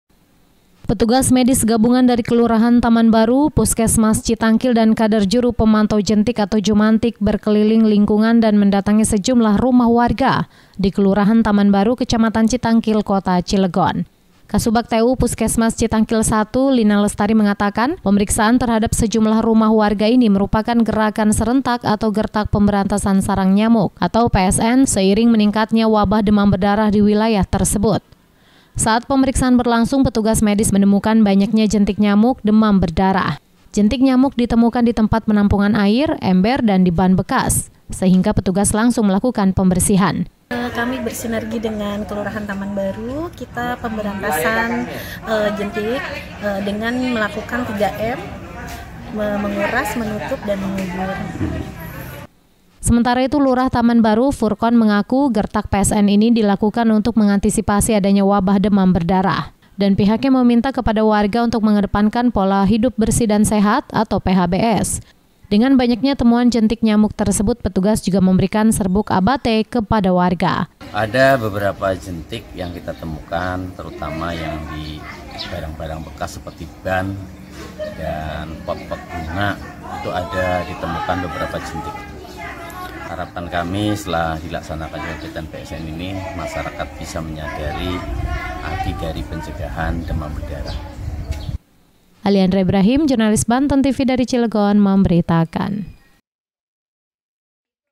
PUSKESMAS CITANGKIL GELAR PROGRAM GERTAK PSN
👇 Baca selengkapnya
Petugas medis gabungan dari Kelurahan Taman Baru, Puskesmas Citangkil dan kader juru pemantau jentik, jumantik melakukan gerakan serentak pemberantasan sarang nyamuk di Kelurahan Taman Baru, Kecamatan Citangkil, Kota Cilegon saat gerakan serentak ini dilakukan, petugas menemukan banyaknya jentik nyamuk di sejumlah rumah warga.